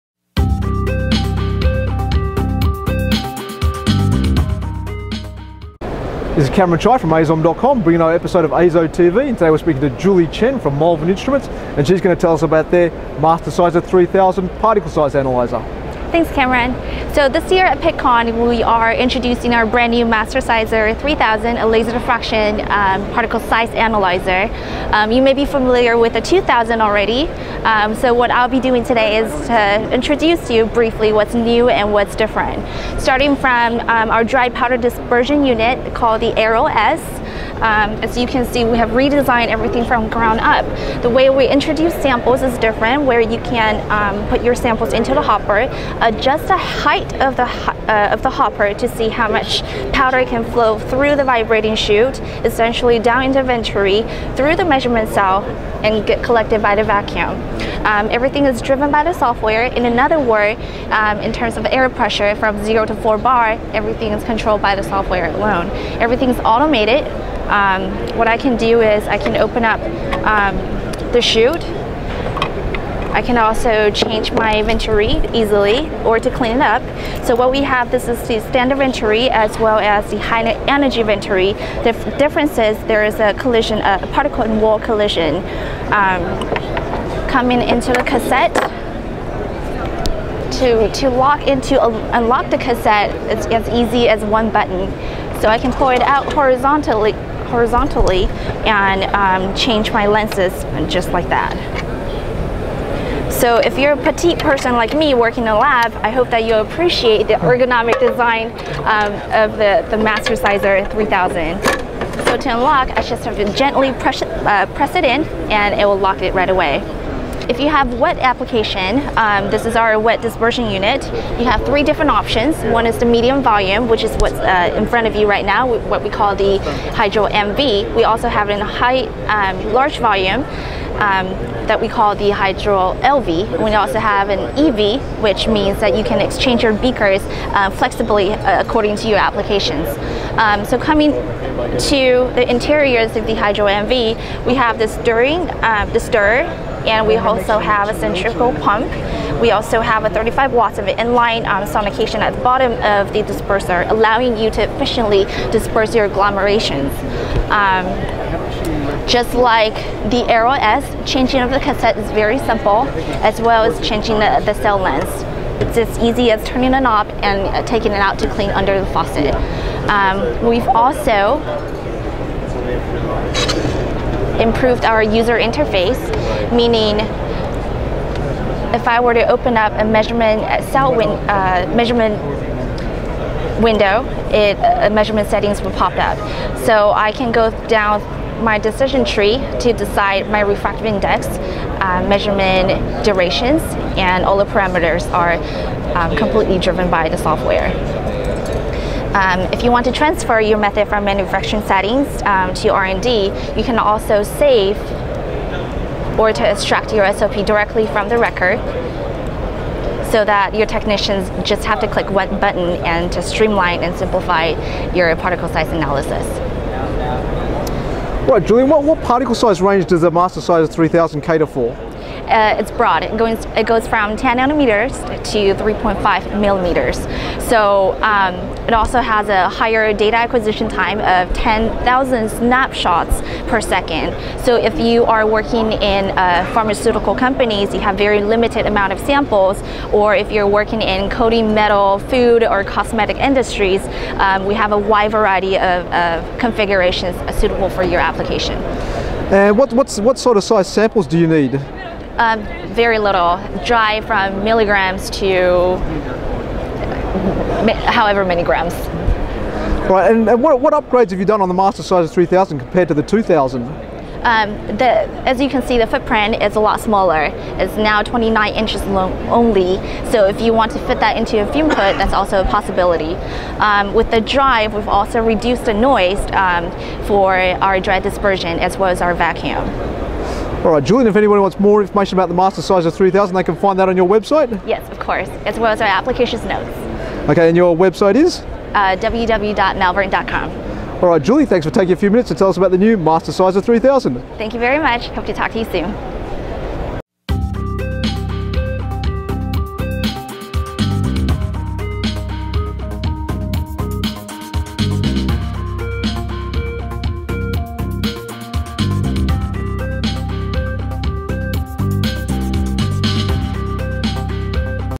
The Malvern Mastersizer 3000 Particle Size Analyzer
Julie Chen from Malvern Instruments shows us Malvern's new Mastyersizer 3000 particle isze analyzer. She outlines upgrades from the Mastersizer 2000 and demonstrates some of the key features and operation of the instrument.